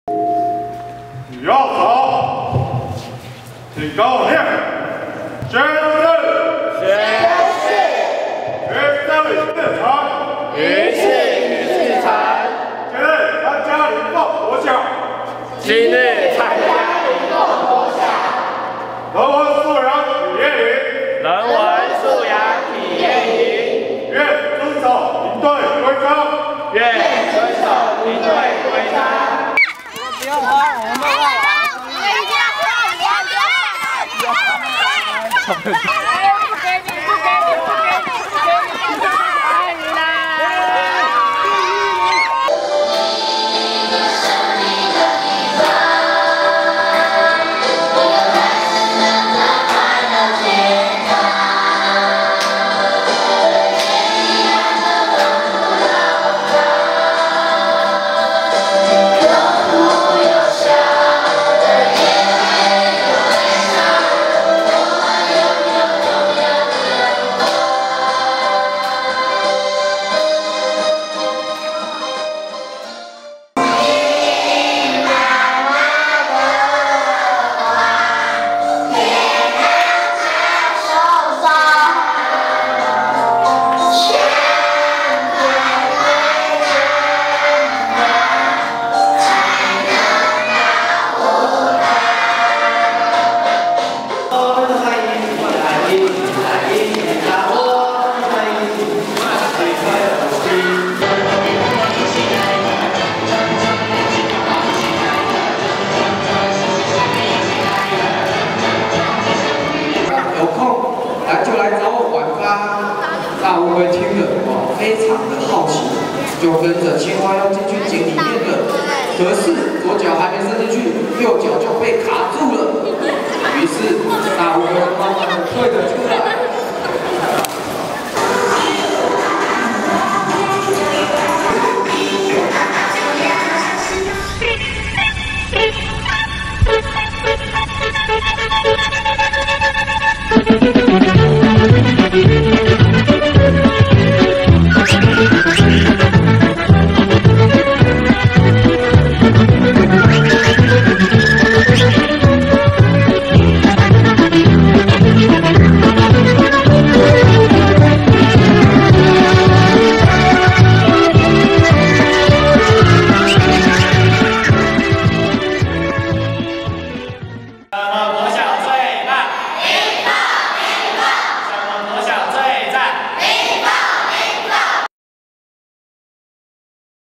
2012-02-01 南台科大社團寒假教育優先區服務志工 前往國小服務成效卓著
一向鼓勵學生積極走出校園，從事社會志工與教育志工服務的南台科技大學，該校光鹽唱詩社與崇德青年社的服務志工們於日前前往台南市西港區成功國小與林鳳國小，展開為期五天的「好寶寶‧品格教育營」與「人文素養成長營」冬令營活動，獲得成功國小與林鳳國小師生們的熱烈參與及肯定！